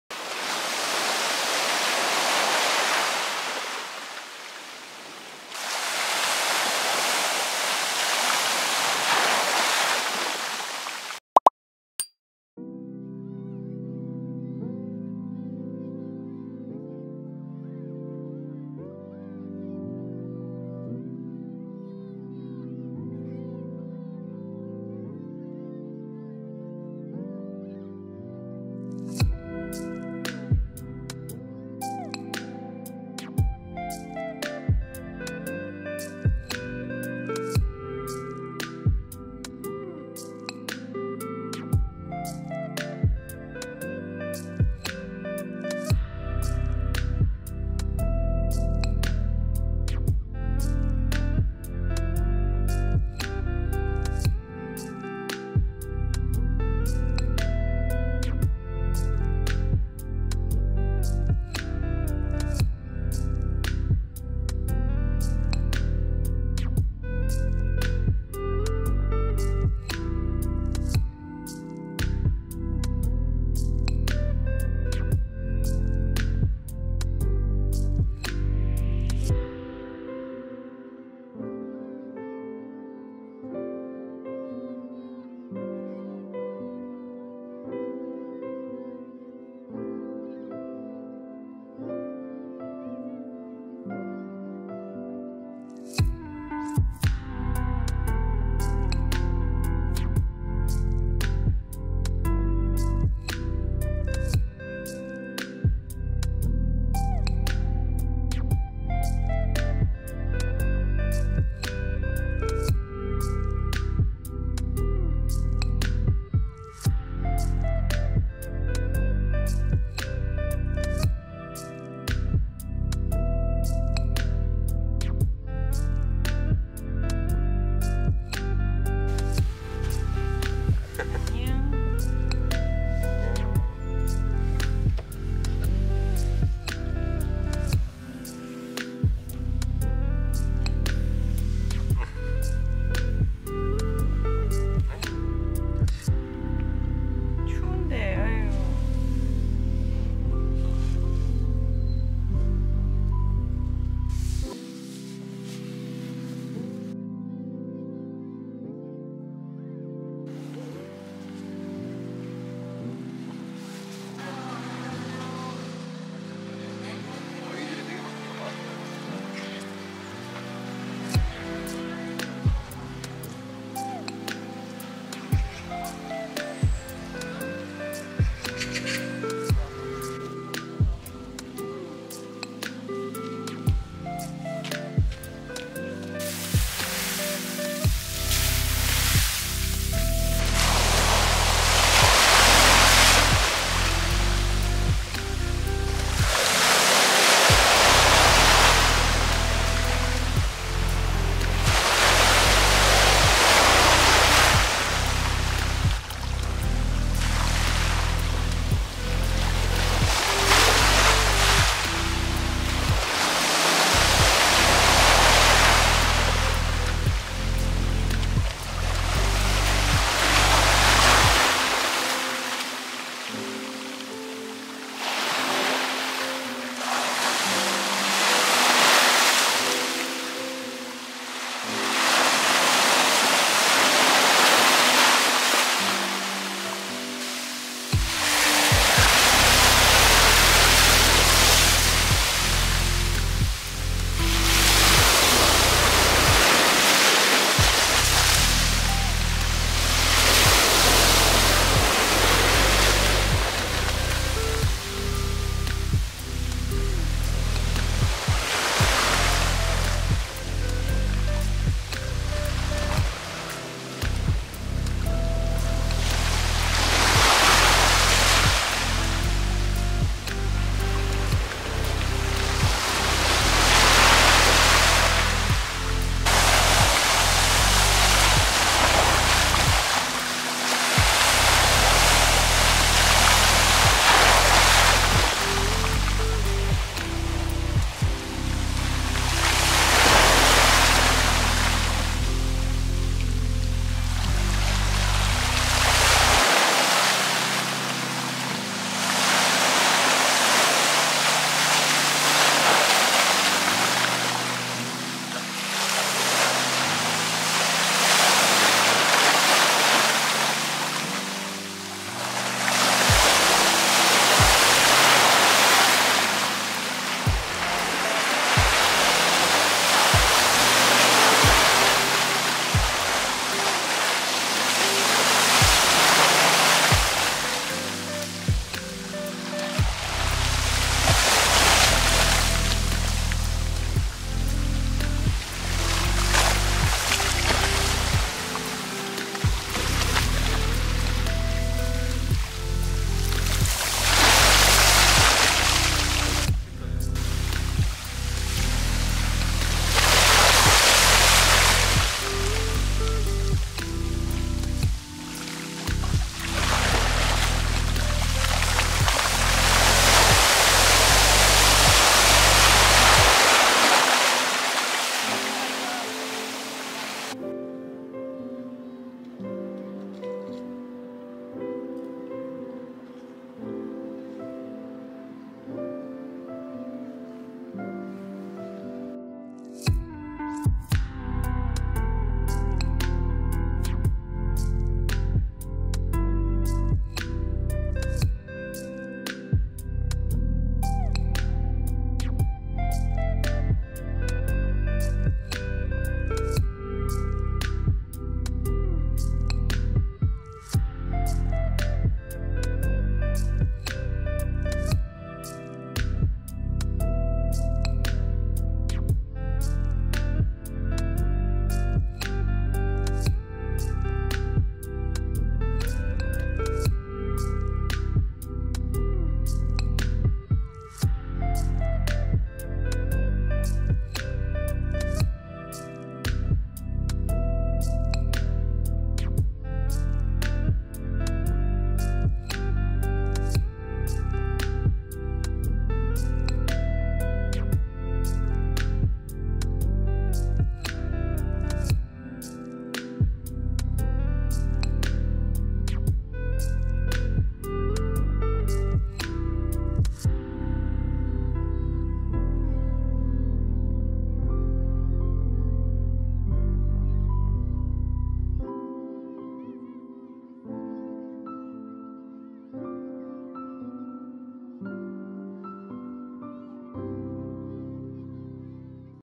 😨 앗! 바다가 얼었다! 🥶 겨울바다_무의도 하나개 해수욕장
바다보러 갔다가 바다가 얼어있더라고요!!😱 요며칠 엄청난 눈과 한파에 바다도 얼어버렸네요~🥶